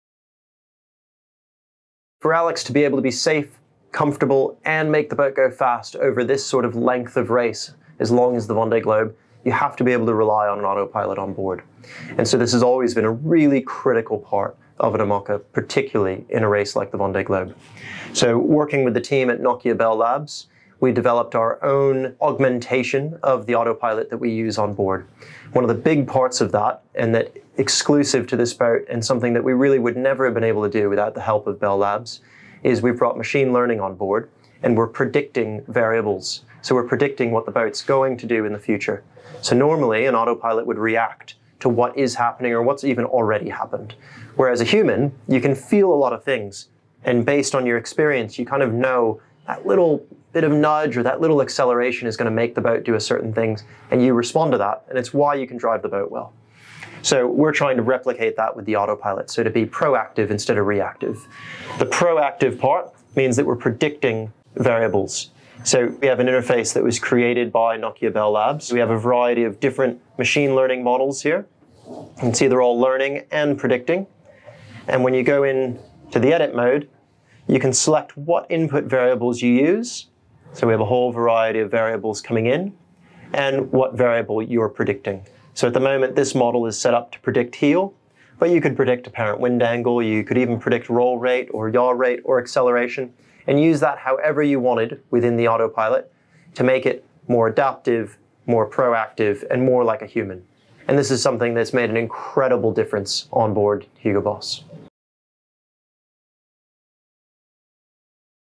Focus: Autopilot
Together with our Technology Partner, Nokia Bell Labs, we have introduced machine learning onboard the HUGO BOSS boat by developing our own augmentation of the autopilot system. Find out more here from our Technical Team Manager, Jesse Rowse.